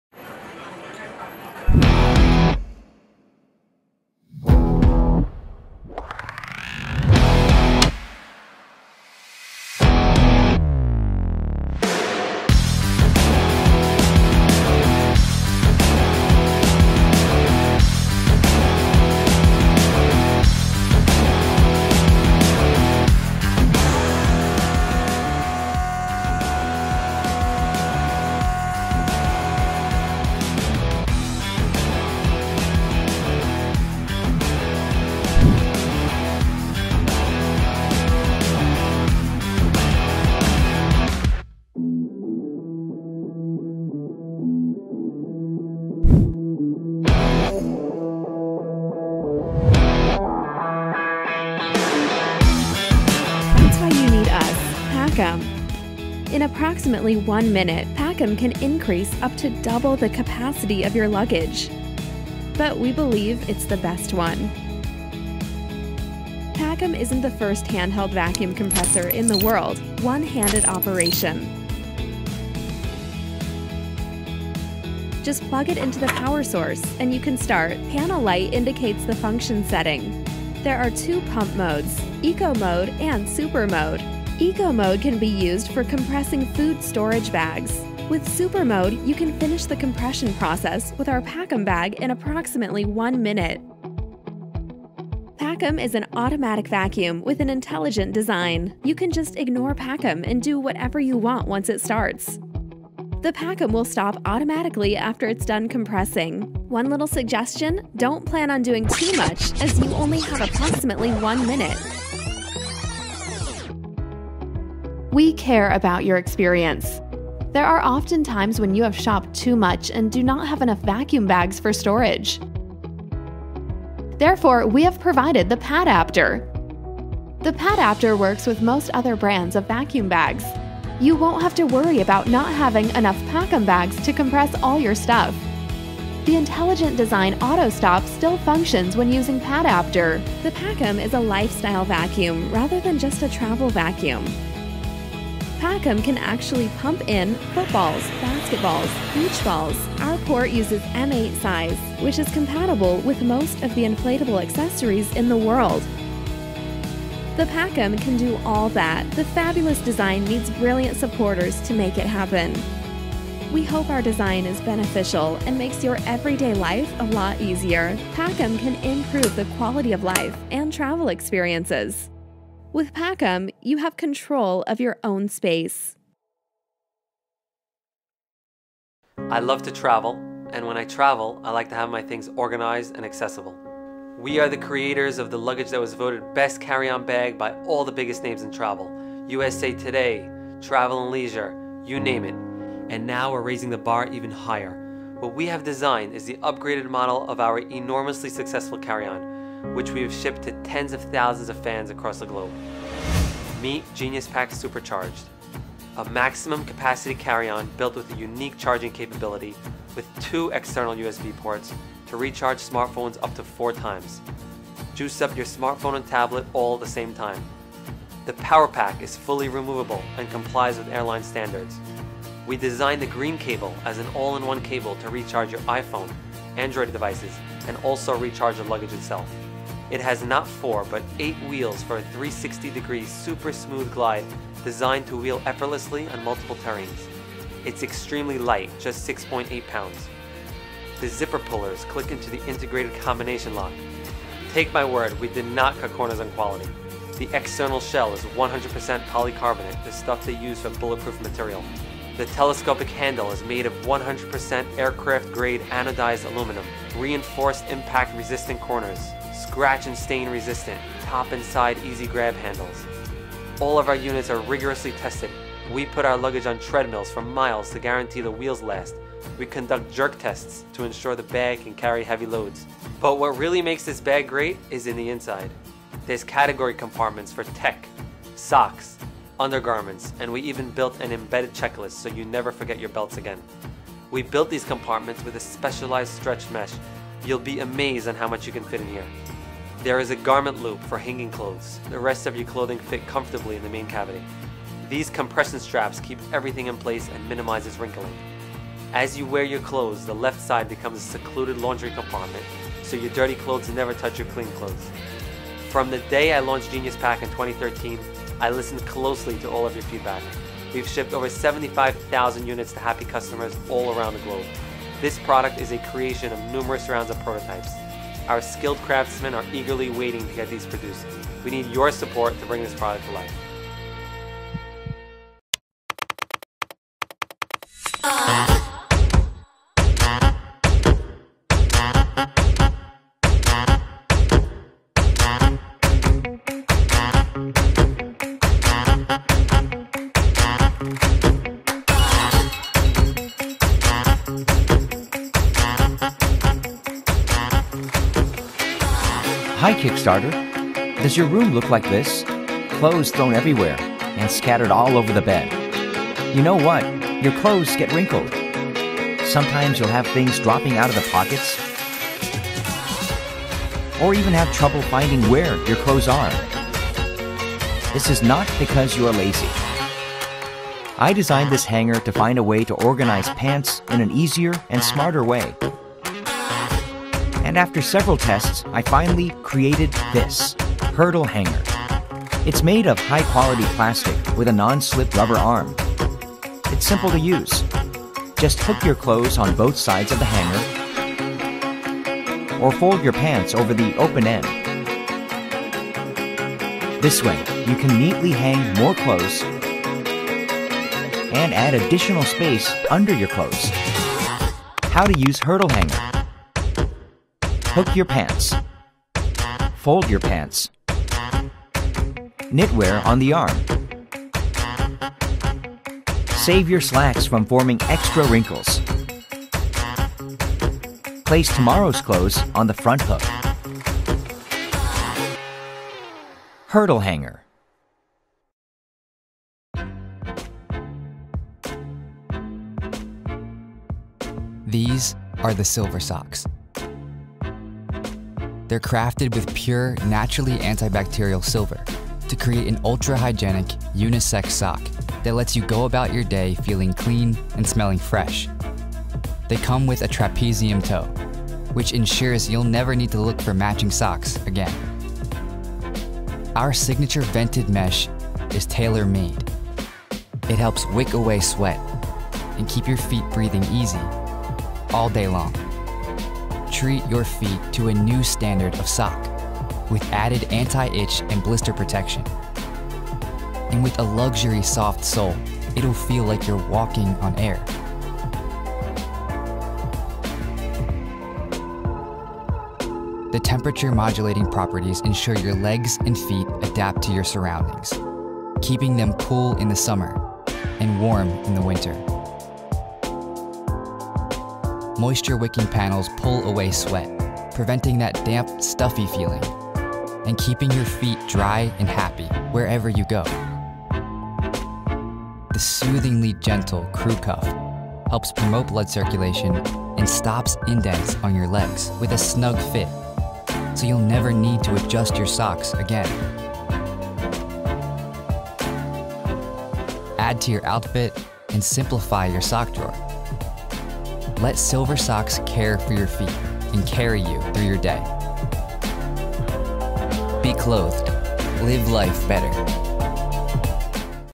Amazing Pacum Vacuum gadget that allows you to pack more luggage in less space.
This video is about the Pacum Vacuum Seal, Silver Comfort Socks, Hurdle Hangars, and The Smart Carry On.  All are amazing cool gadgets and inventions you should look at.